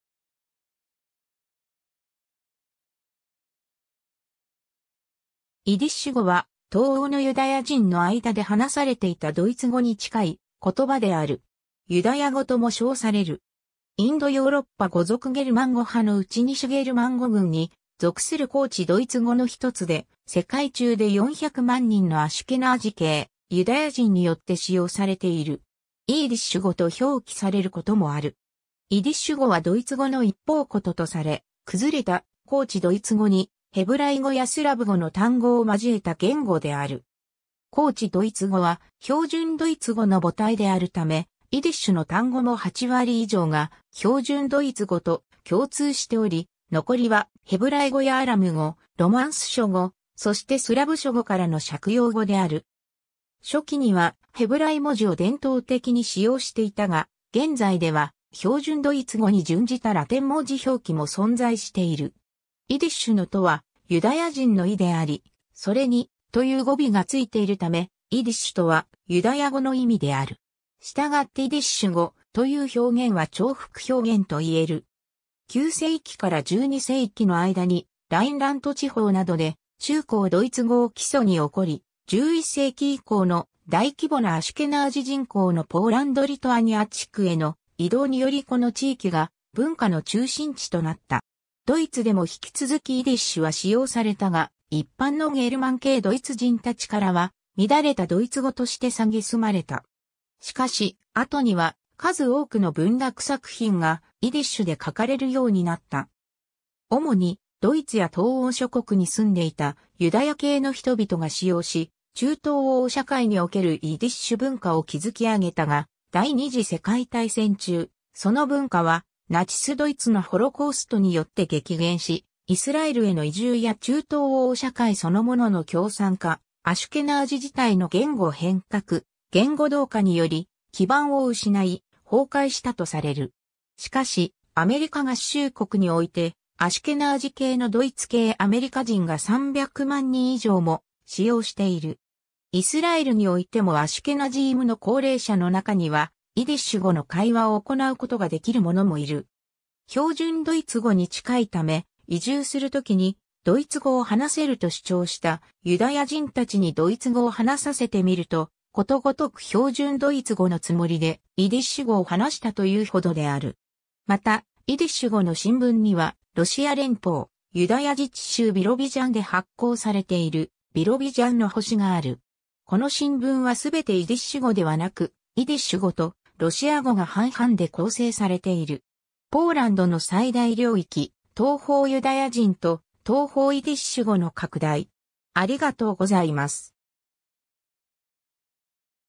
イディッシュ語
危険_(危機に瀕する言語)
イディッシュ語（イディッシュご、イディッシュ語: ייִדיש‎、ドイツ語: Jiddisch、英語: Yiddish、ヘブライ語: ייִדיש‎）は、東欧のユダヤ人の間で話されていたドイツ語に近い言葉である。
ユダヤ語とも称される。
インド・ヨーロッパ語族ゲルマン語派のうち西ゲルマン語群に属する高地ドイツ語の一つで、世界中で400万人のアシュケナージ系ユダヤ人によって使用されている。
イーディッシュ語と表記されることもある。
 イディッシュ語はドイツ語の一方言とされ、崩れた高地ドイツ語にヘブライ語やスラブ語の単語を交えた言語である。
高地ドイツ語は標準ドイツ語の母体であるため、イディッシュの単語も八割以上が標準ドイツ語と共通しており、残りはヘブライ語やアラム語、ロマンス諸語、そしてスラブ諸語からの借用語である。
初期にはヘブライ文字を伝統的に使用していたが、現在では標準ドイツ語に準じたラテン文字表記も存在している。
 イディッシュのייִד（Yid）とはユダヤ人の意であり、それにיש（-ish; 「～語」「～的」）という語尾がついているため、イディッシュとはユダヤ語の意味である（ユダヤドイツ語とも呼ばれる）。
したがってイディッシュ"語"という表現は重複表現（例：サハラ砂漠）といえる。
 9世紀から12世紀の間にラインラント地方などで中高ドイツ語を基礎に興り、11世紀以降の大規模なアシュケナージ人口のポーランド・リトアニア地区への移動によりこの地域が文化の中心地となった。
ドイツでも引き続きイディッシュは使用されたが、一般のゲルマン系ドイツ人たちからは「乱れたドイツ語」として蔑まれた。
しかし、後には数多くの文学作品がイディッシュで書かれるようになった。
 主にドイツや東欧諸国に住んでいたユダヤ系の人々が使用し、中東欧社会におけるイディッシュ文化を築き上げたが、第二次世界大戦中、その文化はナチス・ドイツのホロコーストによって激減し、イスラエルへの移住や中東欧社会そのものの共産化、アシュケナージ自体の言語変革・言語同化により基盤を失い、崩壊したとされる。
しかし、アメリカ合衆国においてアシュケナージ系のドイツ系アメリカ人が300万人以上も使用している。
イスラエルにおいてもアシュケナジームの高齢者のなかにはイディッシュ語の会話を行うことができる者もいる。
 標準ドイツ語に近いため、移住するときに、ドイツ語を話せると主張したユダヤ人たちにドイツ語を話させてみると、ことごとく標準ドイツ語のつもりでイディッシュ語を話したというほどである。
 また、イディッシュ語の新聞には、ロシア連邦ユダヤ自治州ビロビジャンで発行されている『ビロビジャンの星』 (ביראָבידזשאנער שטערן、露: Биробиджанер штерн、独: Birobidschaner Stern)がある。
この新聞はすべてイディッシュ語ではなく、イディッシュ語とロシア語が半々で構成されている。
 ポーランドの最大領域：東方ユダヤ人と東方イディッシュ語の拡大